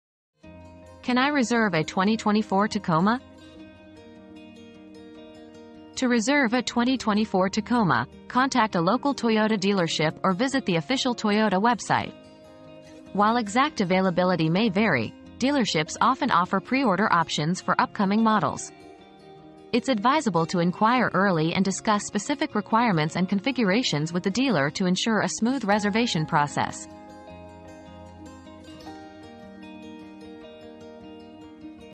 Can I reserve a 2024 Tacoma?
Reserve Your 2024 Tacoma Today! • Reserve Your 2024 Tacoma • Discover how to secure your spot for the highly anticipated 2024 Tacoma. Act fast to discuss your specific requirements and configurations with the dealer for a seamless reservation process.